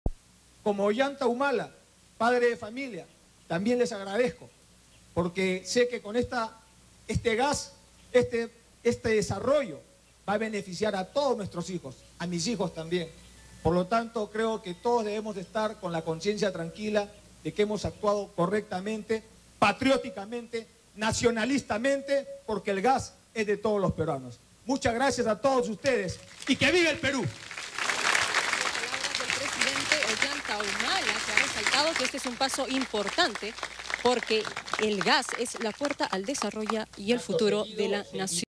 Ollanta en recuperación del lote 88 Camisea: "Hemos actuado patrióticamente, nacionalistamente"
Como Ollanta Humala, padre de familia, también les agradezco porque sé que con este gas, este desarrollo va a beneficiar a todos nuestros hijos, a mis hijos también. Por lo tanto creo que todos debemos estar con la conciencia tranquila de que hemos actuado correctamente, patrióticamente, nacionalistamente, porque el gas es de todos los peruanos. Muchas gracias a todos ustedes y ¡Que viva el Perú!"